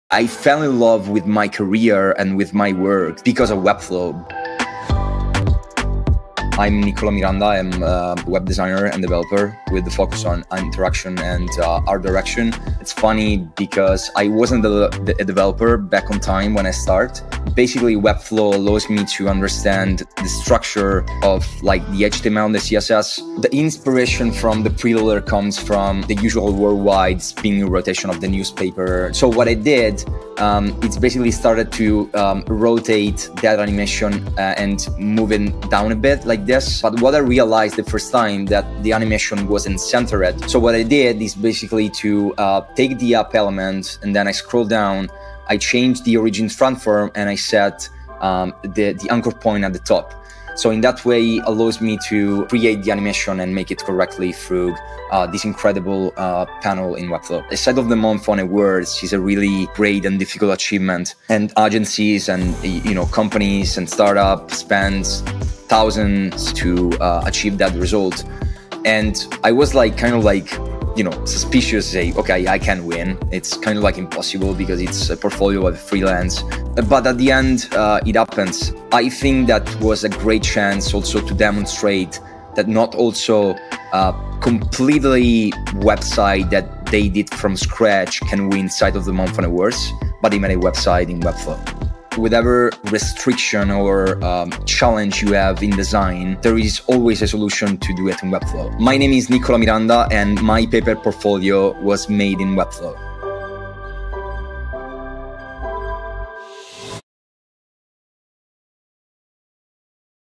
Niccolò Miranda, Paper Portfolio — Made in Webflow
Niccolò Miranda wasn’t a developer when he started designing. But with Webflow, he’s been empowered to flex his skills — claiming his rightful place as a designer and developer. In this episode, Niccolò tells us how he achieved the spiral animation effect on his Paper Portfolio, winner of the Awwwards Site of the Month in November 2021, in just a few clicks.